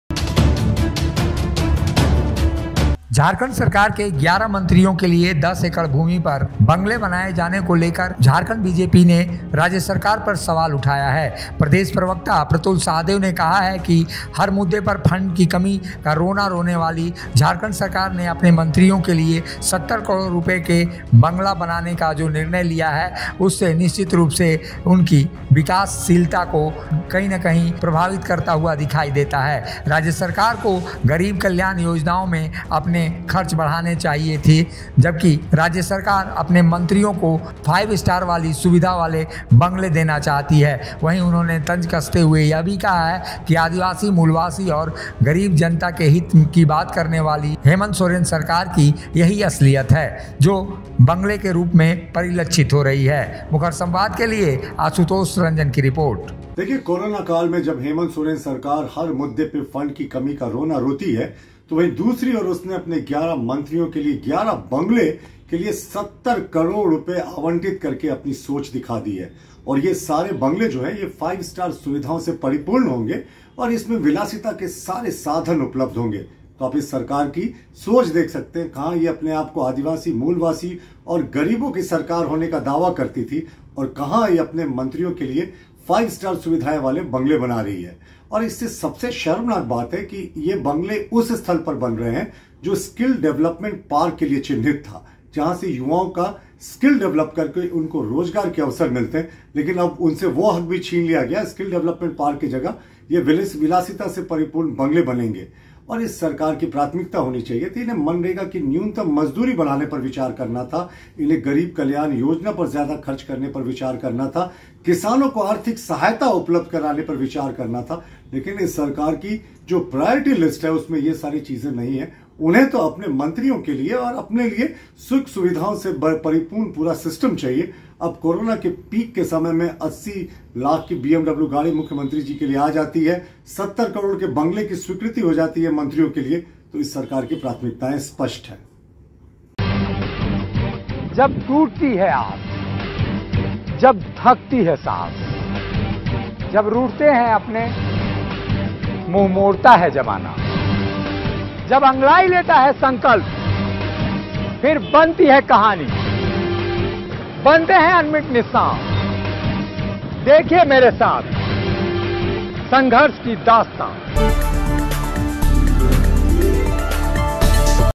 10 एकड़ भूमि पर 11 मंत्रियों के बंगले बनाए जाने पर प्रदेश भाजपा ने राज्य सरकार पर उठाया सवाल
10 एकड़ भूमि पर 11 मंत्रियों के बंगले बनाए जाने पर प्रदेश भाजपा ने राज्य सरकार पर उठाया सवाल